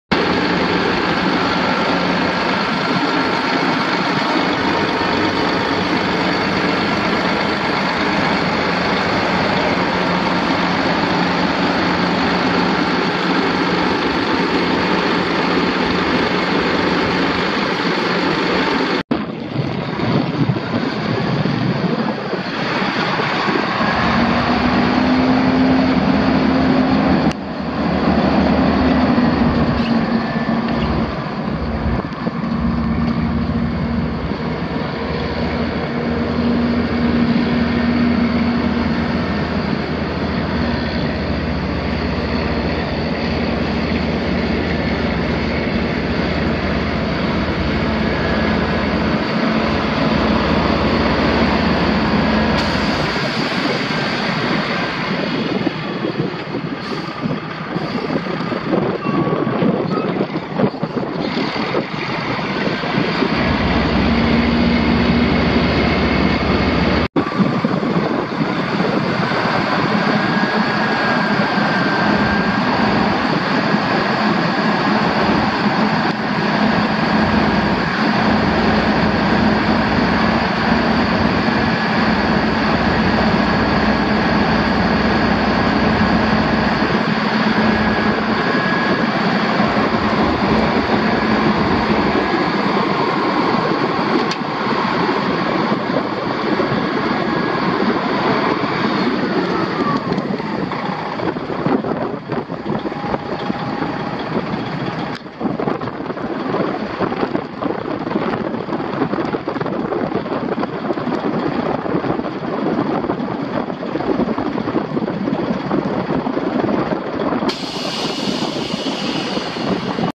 1998 International 9400 dump truck for sale at auction | bidding closes January 25, 2022
1998 International 9400  dump truck
 Miles: 868,727 on odometer
 VIN: 2HSFHAMRXWC053198
Engine
 Detroit Diesel Series 60
 Displacement: 12.7L
 Cylinders: 6
 Fuel type: Diesel
Transmission
 Spicer
 Manual
 Speed: 10
Chassis
 Axles: Tandem
 Suspension: Air
 Brakes: Air
 PTO
Interior
 AC, Heat
 Heated mirrors
 Cruise control
Operational
 Dump bed
 15' L x 94" W
 Bed type: Steel
 Roll tarp
 Ram: Single
 End gate: Manual
Tires
 Size: 11R22.5
Notes
 Dash warning indicator: Check engine
Oklahoma title
It's located in Newkirk, OK, and we're selling it to the highest bidder regardless of price.